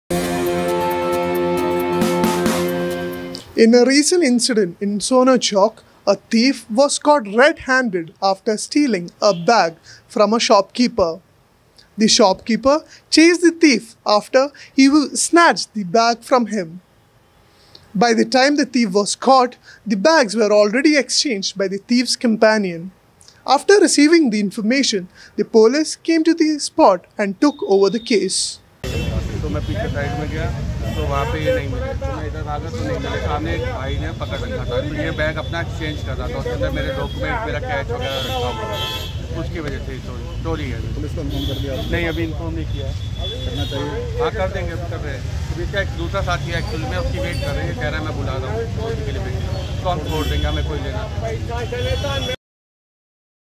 HARYANA AHEAD: A thief was caught red handed while stealing in Sohna Chowk
In an incident reported in Sona Chowk, a thief was caught red-handed while stealing a bag and was tied up against a tree. The shopkeeper chased the thief after the thief snatched the bag from him. By the time the thief was caught, the bags were exchanged by the thief's companion.
After receiving the information the police came to the spot and took over the case.